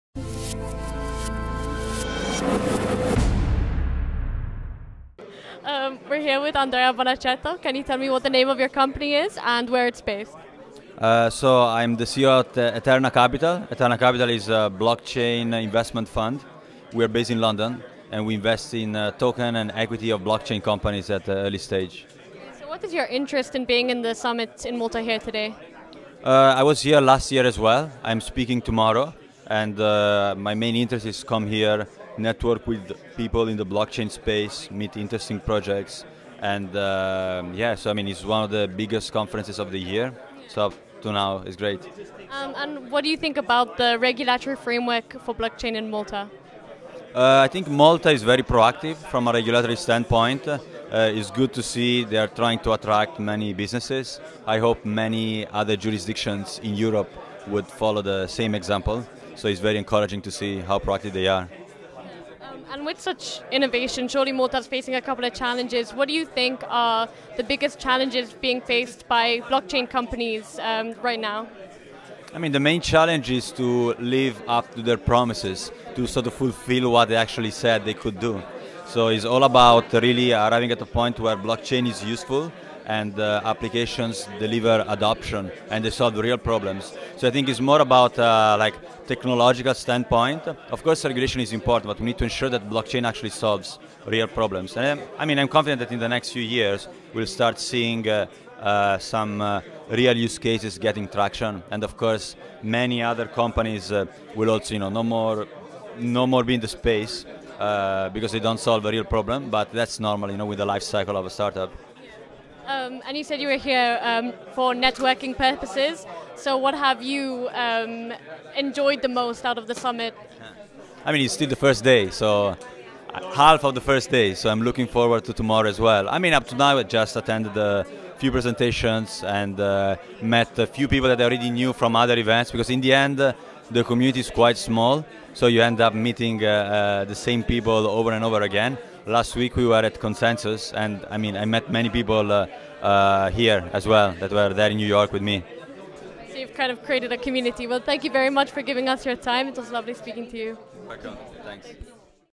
Andrea Bonaceto will tell us about Eterna Capital and Blockchain Technology
Andrea Bonaceto is the Founder & CEO of Eterna Capital. He is going to tell us about this company and the regulations on Blockchain in Malta.

Eterna Capital is an investment company focused on blockchain technology. It's poised to play a key role in shaping the future of blockchain technology by supporting its growth while giving its investors access to the market. They believe that blockchain technology has the ability to disrupt most industries around the globe and that such disruption will have significant impact on society in the long-term.